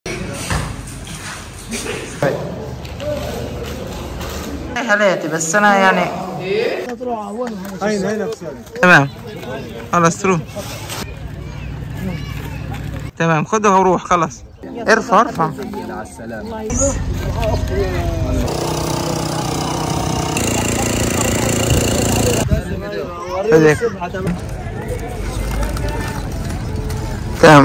Food Parcel Distribution in Gaza - 26th June 2024
Gaza is in Need of Food

The people in Gaza are currently experiencing severe food shortages.